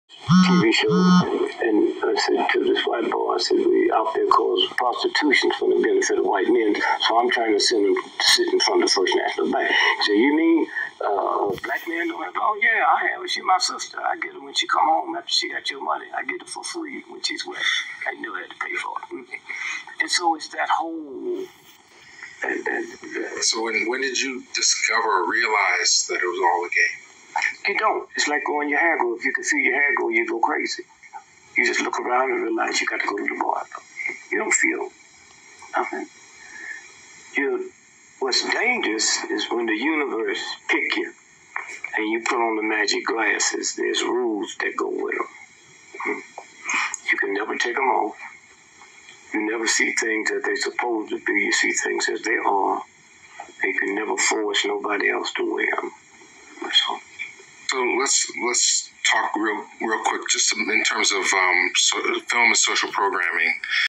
Dick Gregory You never see things the same again
Dick Gregory exposes the lies reality isn't what you really think 🤔